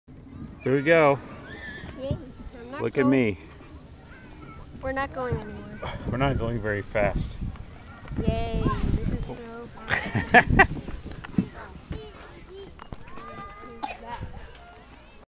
Big Slide, Slow RIde
Ken and Nathaniel having some fun at Robins Farm, Arlington, MA